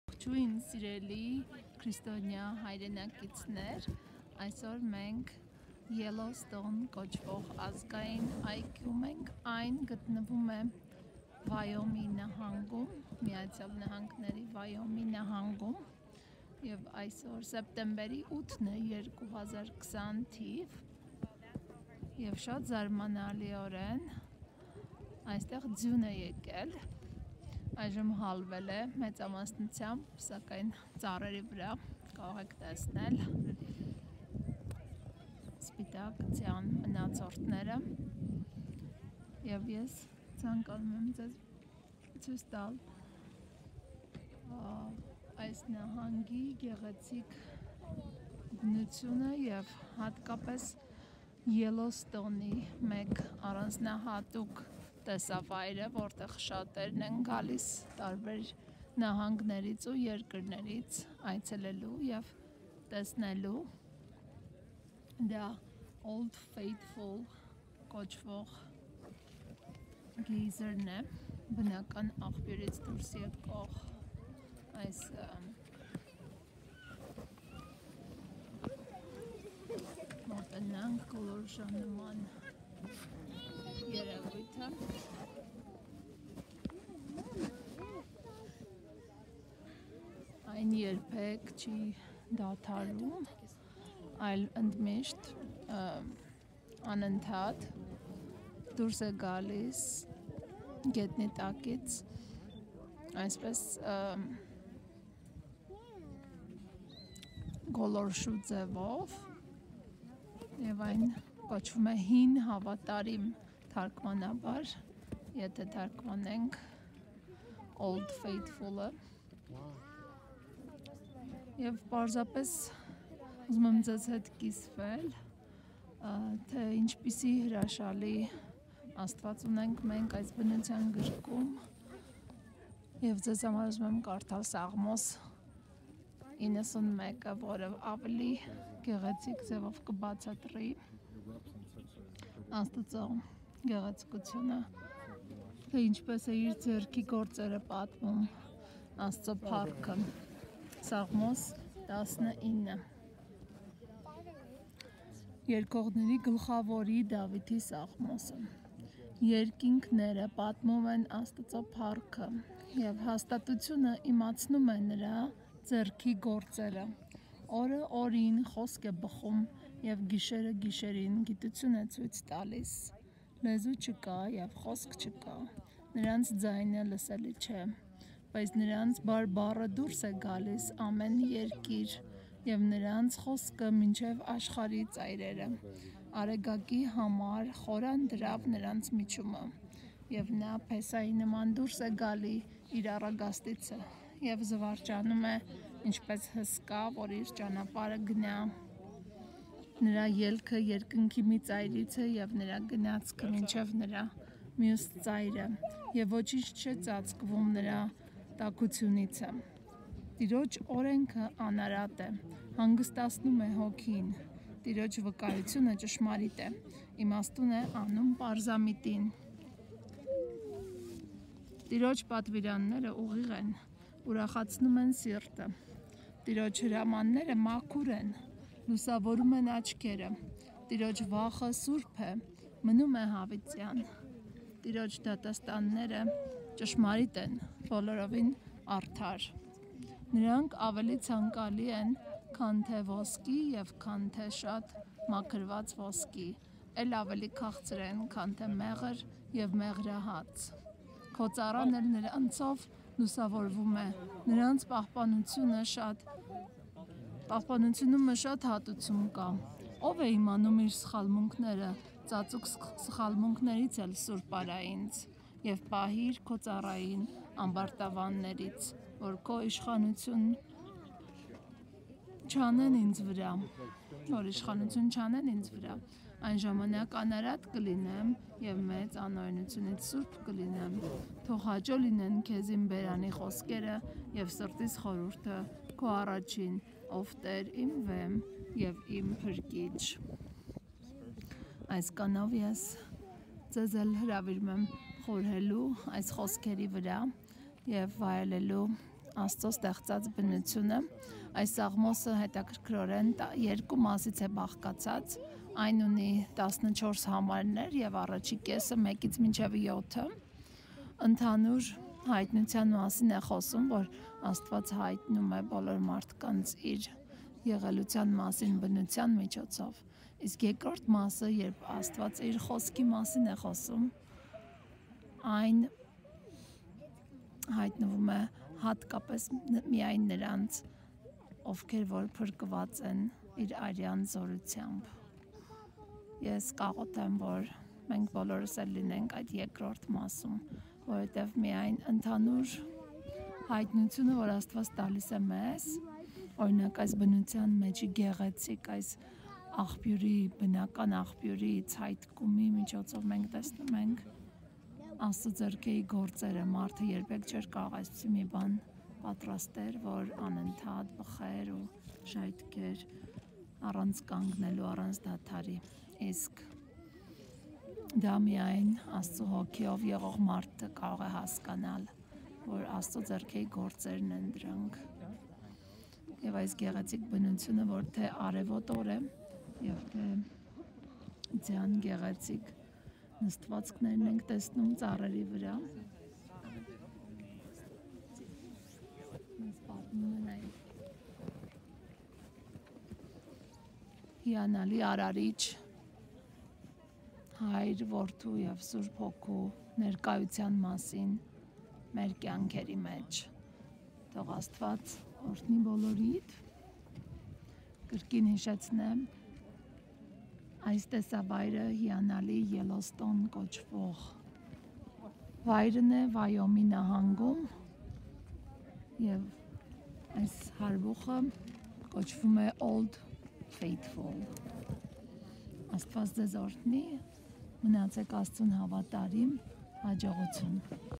Սաղմոս 19 and Old Faithful in Yellowstone
ՍԱՂՄՈՍ 19
1 Երգողների գլխաւորին։ Դավիթի Սաղմոս։
1 Երկինքները պատմում են Աստուծոյ փառքը, եւ հաստատութիւնը իմացնում է նորա ձեռքի գործքերը։
2 Օրը օրին խոսք է բղխում. Եւ գիշերը գիշերին գիտութիւն է ցոյց տալիս։
3 Լեզու չ’կայ եւ խոսք չ’կայ. Նորանց ձայնը լսելի չէ.
4 Բայց նորանց բարբառը դուրս է գալիս  ամեն երկիր, եւ նորանց խոսքը մինչեւ աշխարհքի ծայրերը. Արեգակի համար խորան դրաւ նորանց միջումը։
5 Եւ նա փեսայի նման դուրս է գալիս իր առագաստիցը, եւ  զուարճանում է ինչպէս հսկայ որ իր ճանապարհը գնայ։
6 նորա ելքը երկնքի մի ծայրիցը, եւ նորա գնացքը մինչեւ նորա միւս ծայրը. Եւ ոչինչ չէ ծածկվում նորա տաքութիւնիցը։
7  Տիրոջ օրէնքը անարատ է, հանգստացնում է հոգիին. Տիրոջ վկայութիւնը ճշմարիտ է, իմաստուն է անում պարզամիտին։
8  Տիրոջ պատուիրանքները ուղիղ են, ուրախացնում են սիրտը. Տիրոջ հրամանները մաքուր են,  լուսաւորում են աչքերը։
9 Տիրոջ վախը սուրբ է, մնում է յաւիտեան. Տիրոջ դատաստանները ճշմարիտ են, բոլորովին արդար։
10 Նորանք աւելի ցանկալի են քան թէ ոսկի եւ քան թէ շատ մաքրուած ոսկի. էլ աւելի քաղցր են քան թէ մեղր եւ մեղրահաց։
11 Քո ծառան էլ նորանցով լուսաւորվում է.  նորանց պահպանութիւնումը շատ հատուցում կայ։
12  Ո՞վ է իմանում իր սխալմունքները.  ծածուկ սխալմունքներիցս էլ լ սուրբ արա ինձ։
13  Եւ պահիր քո ծառային ամբարտաւաններից, որ իշխանութիւն չ’անեն ինձ վերայ. Այն ժամանակ անարատ կ’լինիմ եւ մեծ անօրէնութիւնից սուրբ կ’լինիմ։
14  Թող հաճոյ լինին քեզ իմ բերանի խօսքերը. Եւ սրտիս խորհուրդը՝ քո առաջին, ով Տէր, իմ վէմ եւ իմ Փրկիչ։